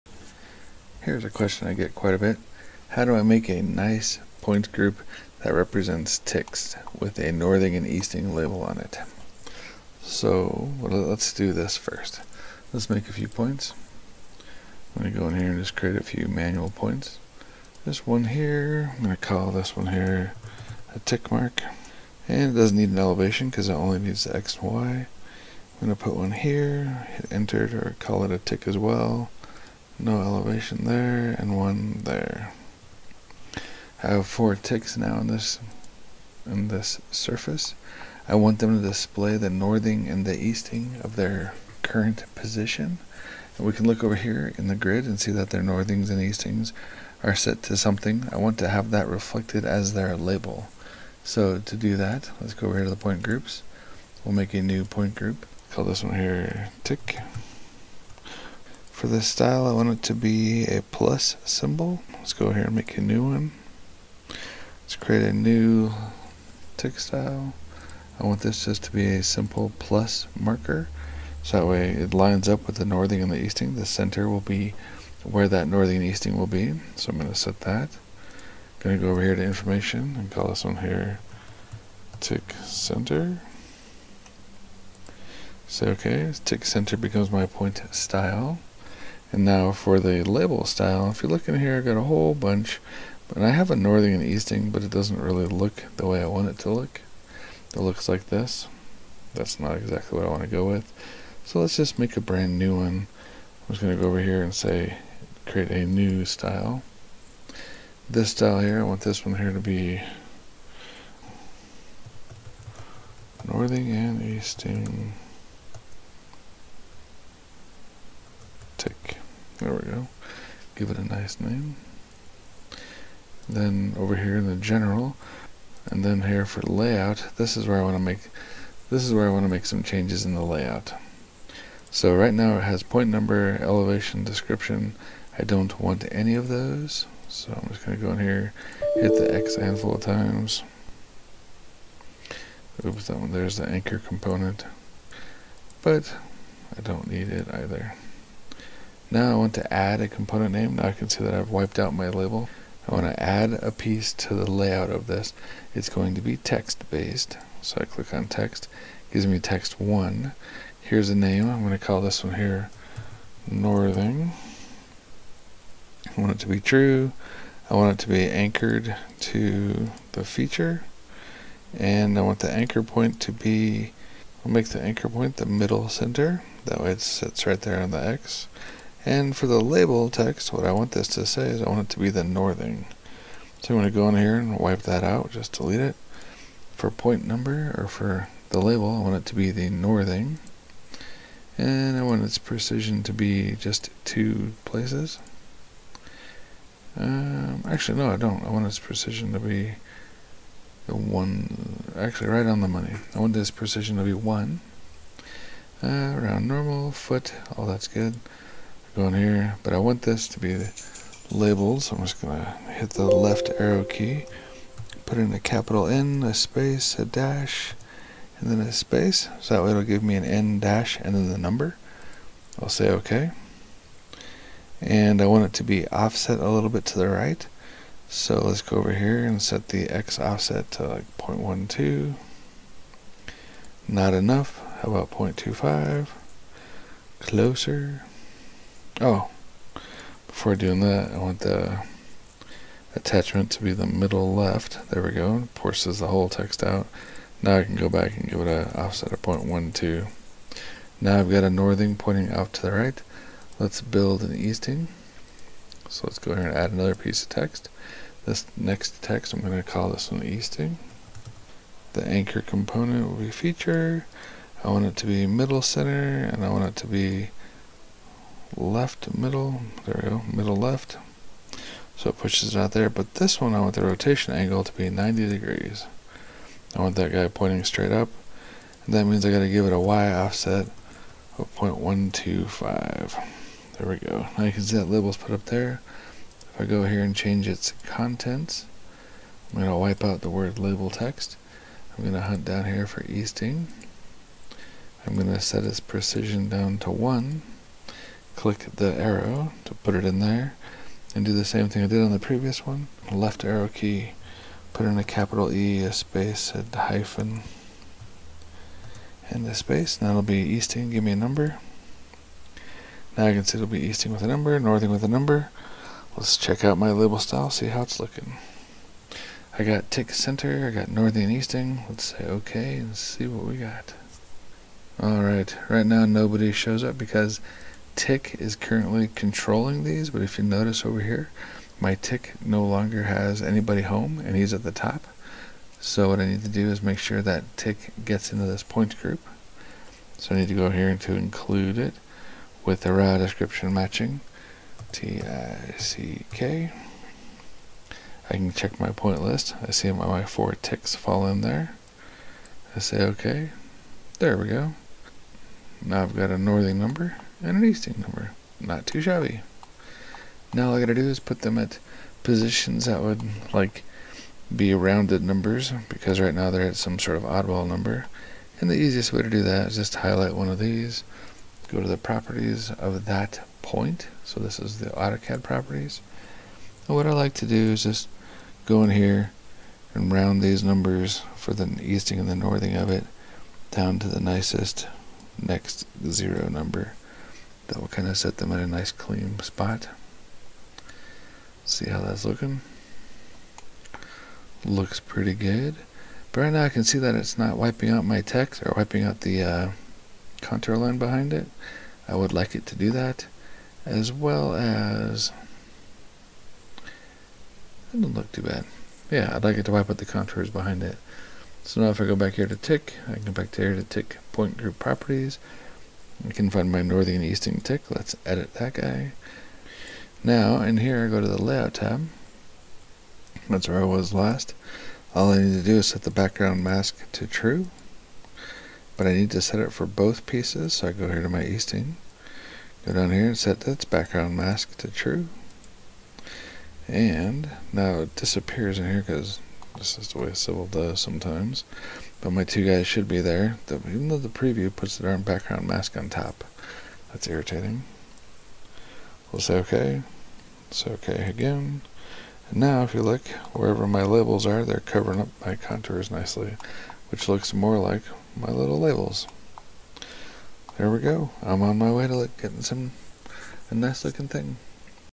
Civil 3D - Point Label Style - Northing and Easting
Making a custom point style in Autodesk's Civil 3D for a group to represent Northing and Easting Tick marks.